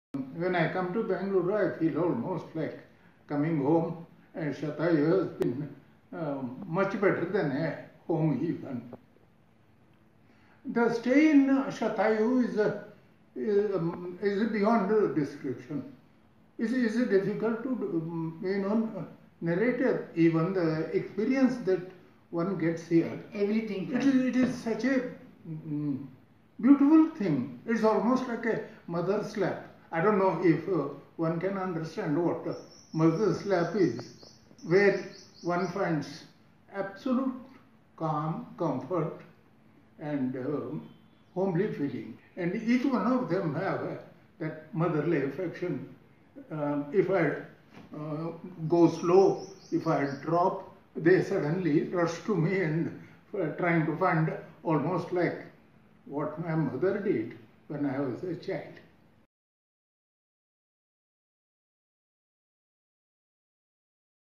Journey of an elderly couple | Shathayu Ayurveda Yoga Retreat
Retired Army officer Major Suresh Galgali and Mrs.Usha Galgali talk about their wonderful experience with the "Elderly Health Care Program" @ ShathayuRetreat